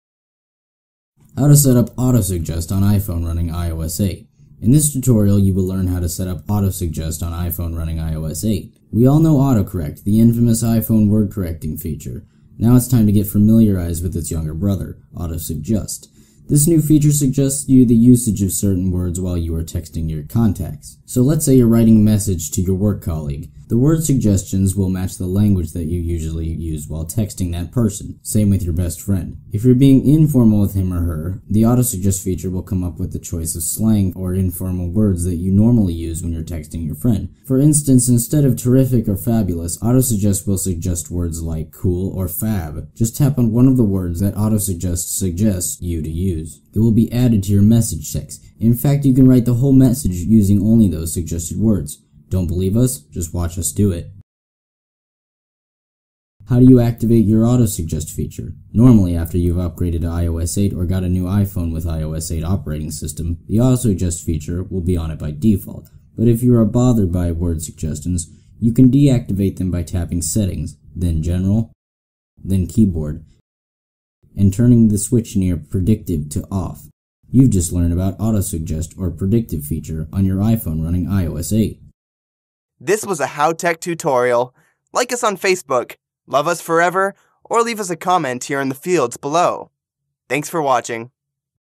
How to Setup Autosuggest on iPhone and iPad
In this tutorial, you will learn how to setup Autosuggest on iPhone.

We all know Autocorrect  - the infamous iPhone word-correcting feature. Now it’s time to get familiarized with its younger brother - Autosuggest. This new feature suggests you the usage of certain words while you are texting your contacts.

Step 1.  So, let’s say you’re writing a message to your work colleague. The word suggestions will match the language that you usually use while texting that person. If you’re being informal with him or her, the Autosuggest feature will come up with the choice of slang or informal words that you normally use when texting your friend. For instance, instead of “terrific” or “fabulous”, Autosuggest will suggest you words like “cool” or “fab”.

Step 2. Just tap on one of the words that Autosuggest suggests you to use. It will be added to your message text.

Step 3. In fact, you can write the whole message using only those suggested words. Don’t believe us? Just watch us do it.

Step 4. How do you activate your Autosuggest feature? Normally, after you’ve upgraded to iOS8 or got a new iPhone with iOS8 operating system, the Autosuggest feature will be ON by default. But if you are bothered by word suggestions, you can deactivate them by tapping “Settings”, then “General”, then “Keyboard” and turning the switch near “Predictive” to off.

Result: You’ve just learned about Autosuggest or “predictive” feature on your iPhone running on iOS8.